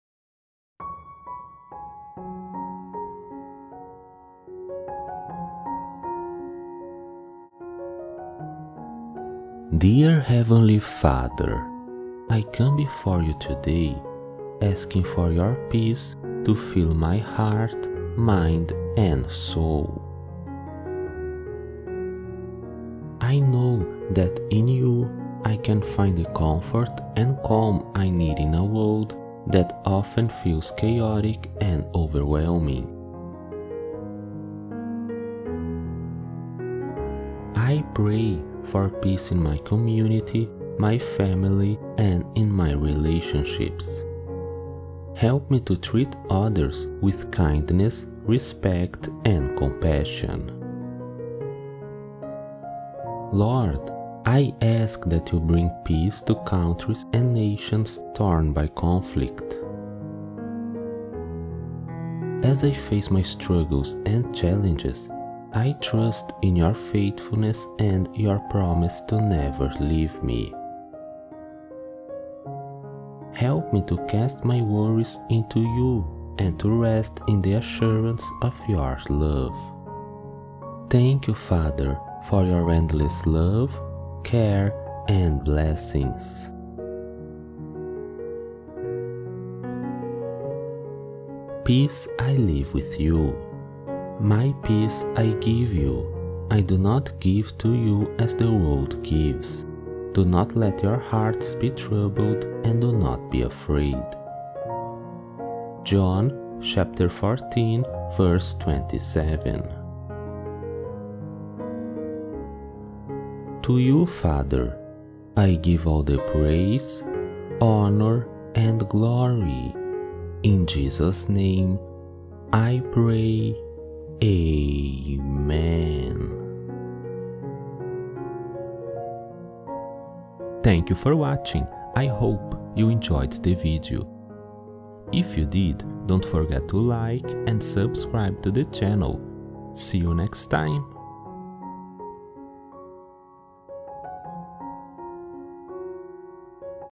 A Short Prayer for Peace - God, help me to cast my worries into you - Daily Prayers #719
Dear Heavenly Father, I come to you today asking for your peace to fill my heart, mind, and soul. I know that in you, I can find the comfort and calm I need in a world that often feels chaotic and overwhelming. I pray for peace in my community, in my family, and in my relationships. Help me to treat others with kindness, respect, and compassion. Lord, I ask that you bring peace to countries and nations torn by conflict. As I face my struggles and challenges, I trust in your faithfulness and your promise to never leave me. Help me to cast my worries into you, and to rest in the assurance of your love. Thank you Father for your endless love, care and blessings.

"Peace I leave with you; my peace I give you. I do not give to you as the world gives. Do not let your hearts be troubled and do not be afraid." (John 14:27)

To you Father, I give all the praise, honor and glory. In Jesus name I pray. Amen.